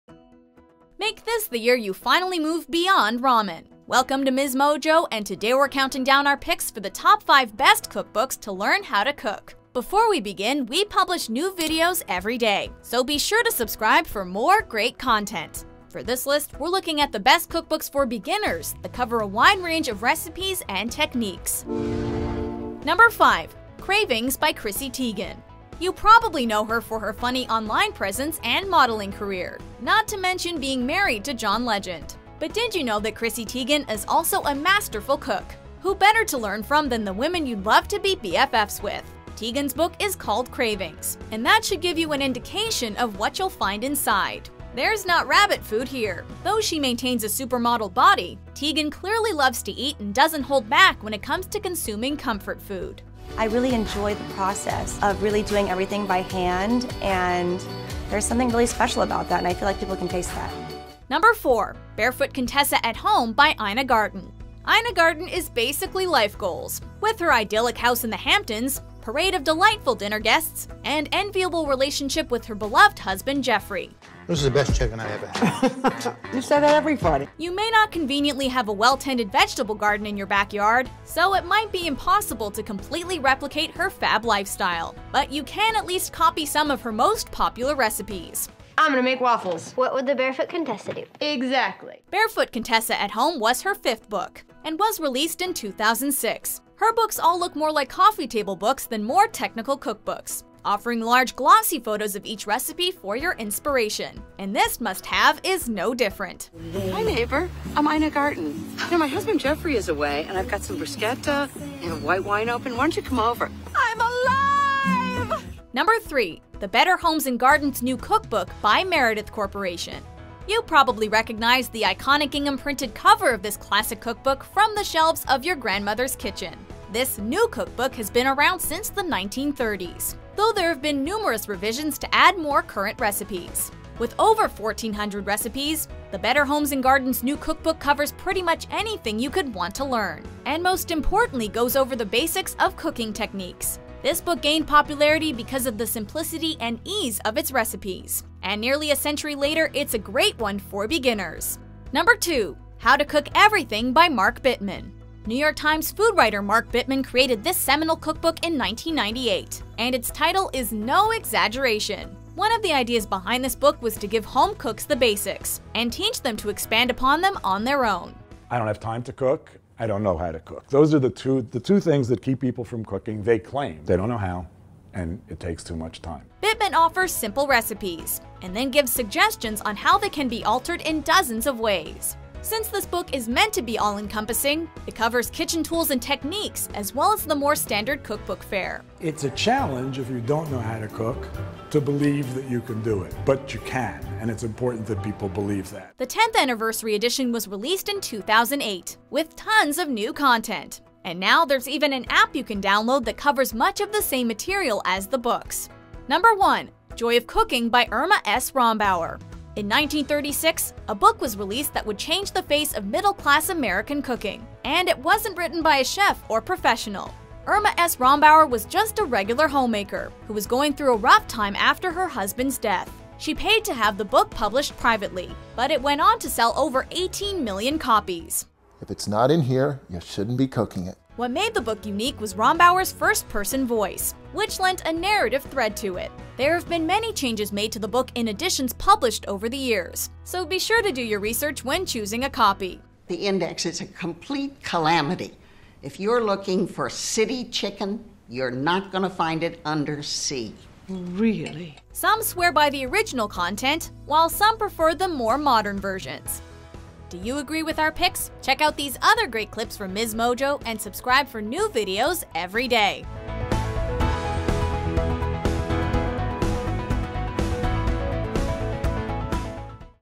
Top 5 Must Own Cookbooks
Top 5 Best Cookbooks to Learn How to Cook

Want to learn how to cook and don’t know where to start? There are so many cookbooks out there, where do you begin? Here are Top 5 cookbooks we highly recommend if you’re looking to learn! We’ve included cookbooks such as “Cravings” by Chrissy Teigen, “Barefoot Contessa at Home” by Ina Garten, ““The Better Homes and Gardens New Cookbook” by Meredith Corporation, and “How to Cook Everything” by Mark Bittman.